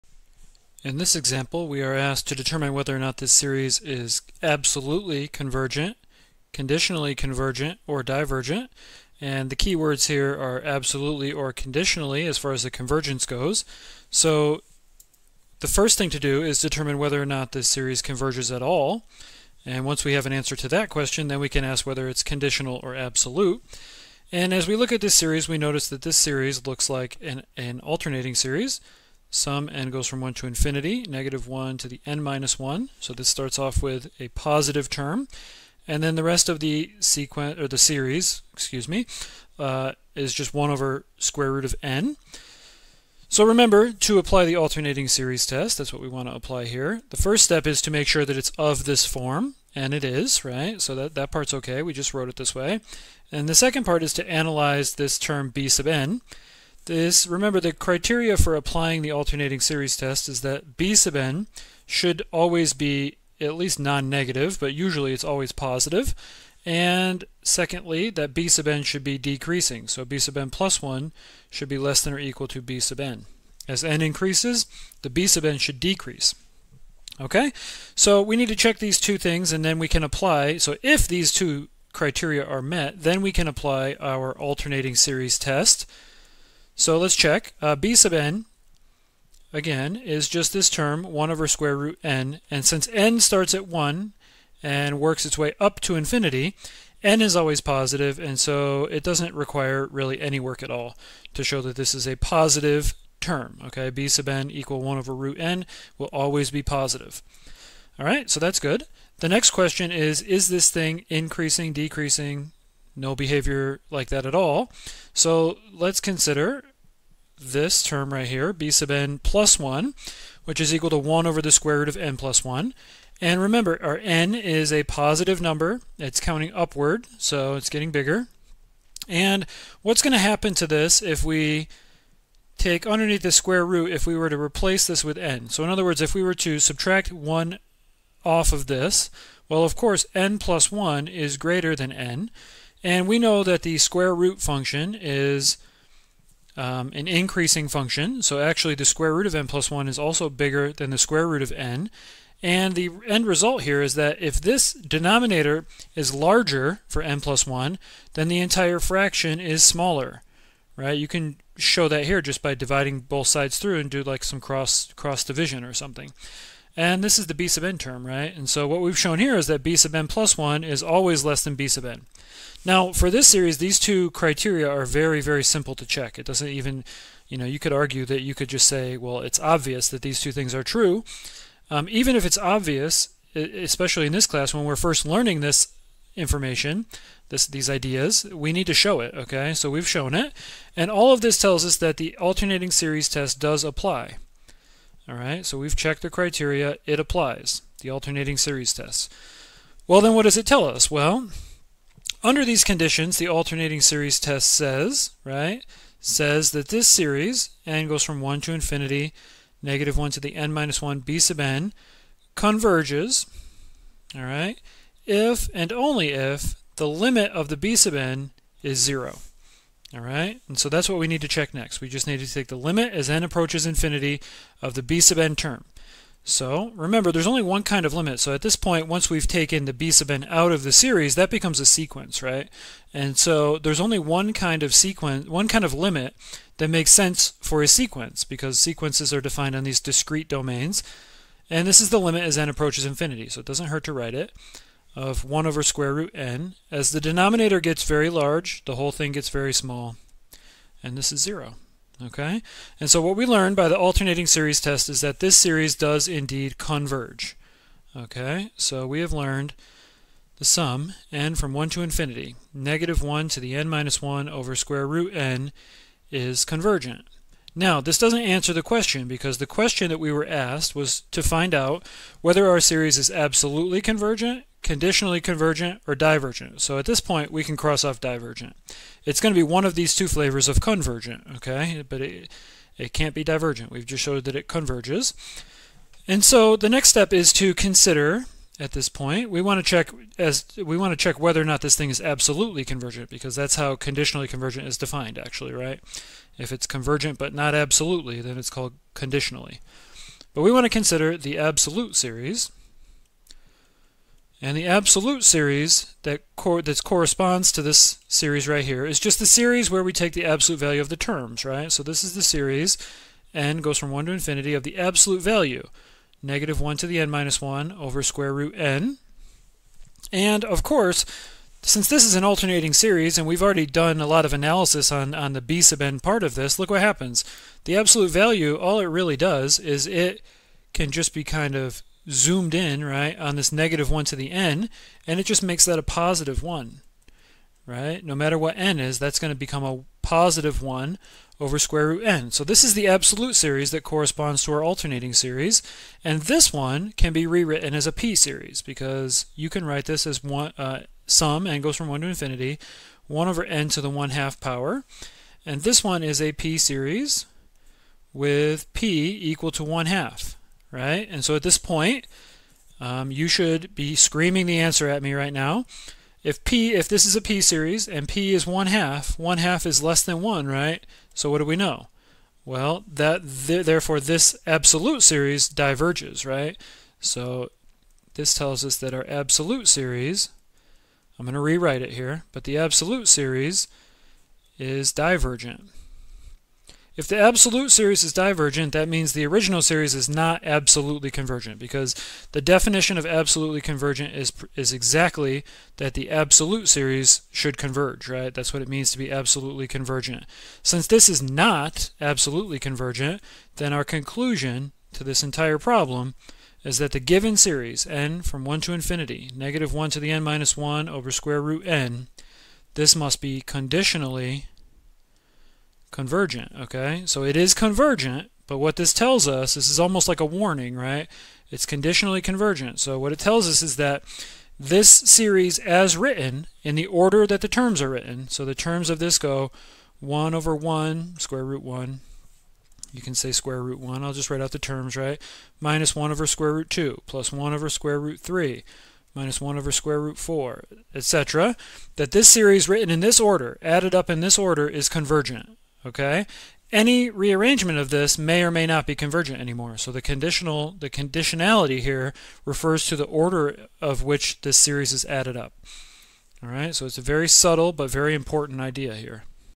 Example: Conditional versus Absolute Convergence
We examine a series to determine whether it is conditionally convergent, absolutely convergent, or divergent.